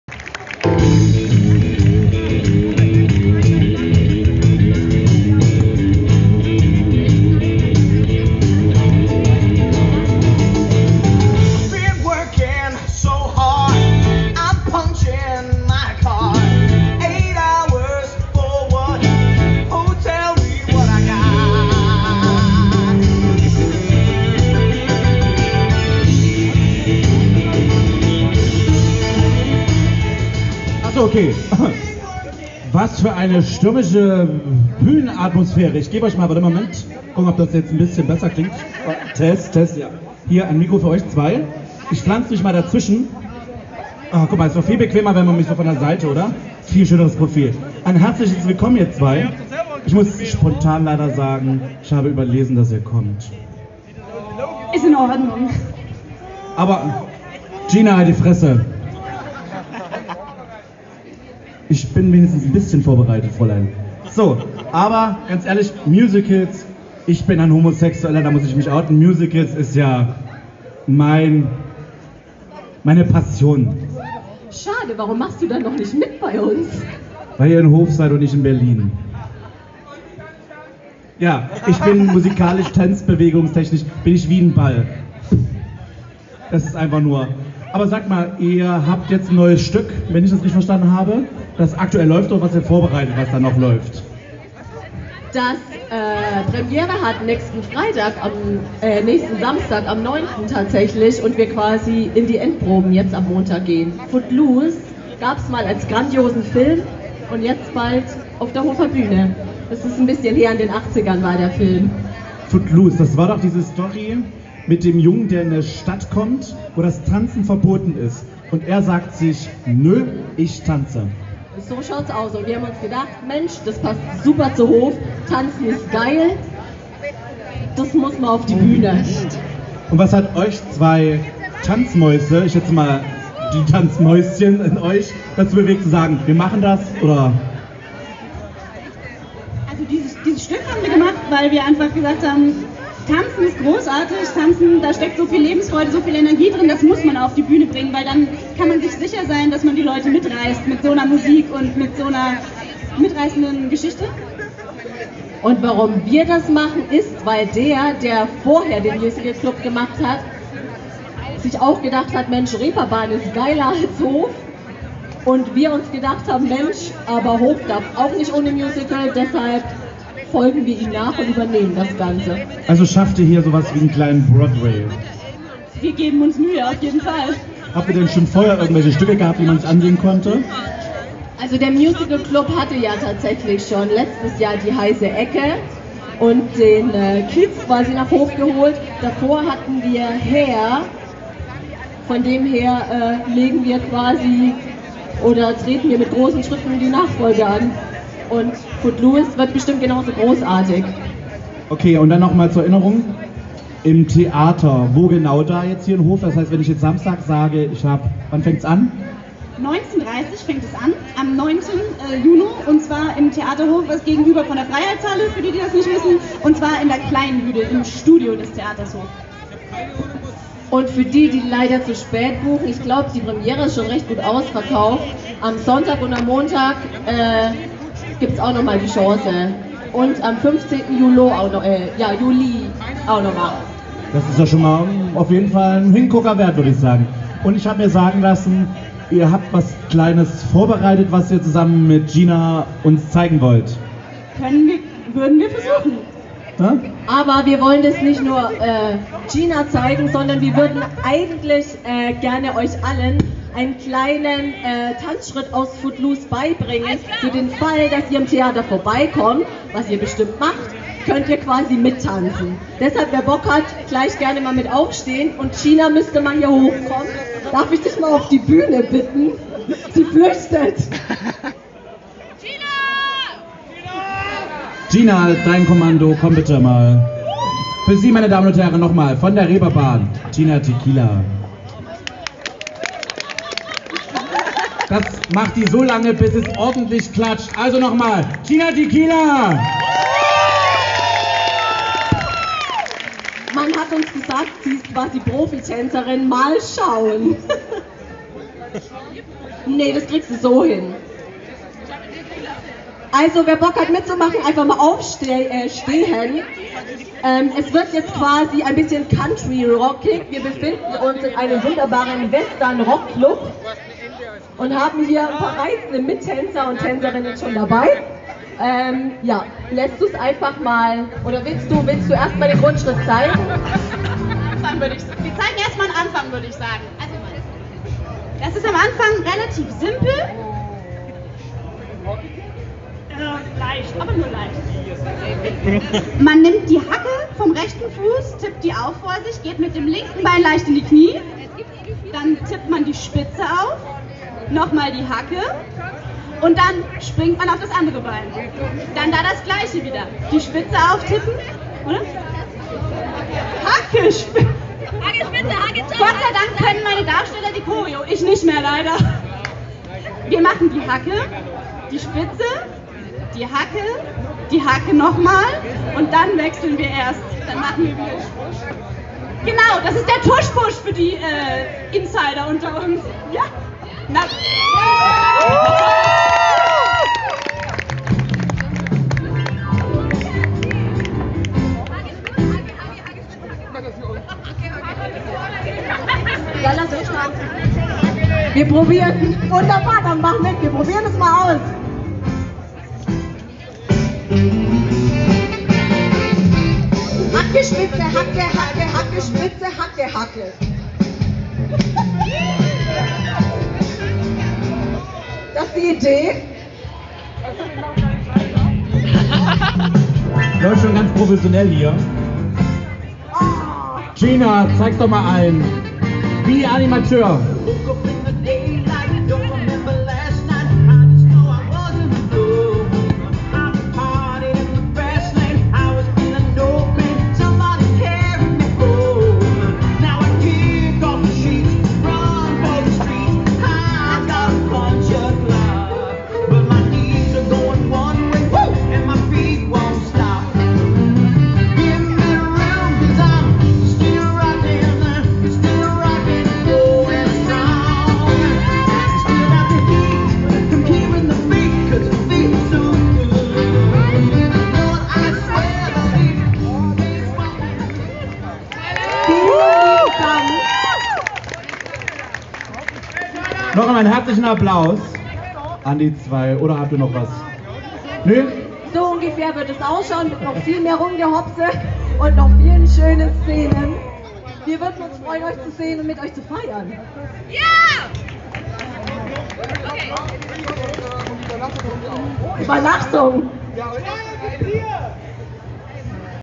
Queerbeat auf dem 3. Hofer Stadt- und Bürgerfest am 02.06.2018 - Teil 13
Queerbeat auf dem 3. Hofer Stadt- und Bürgerfest am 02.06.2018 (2. inoffizieller CSD Hof) - Teil 13 - Interview mit Musicalclub des Theater Hof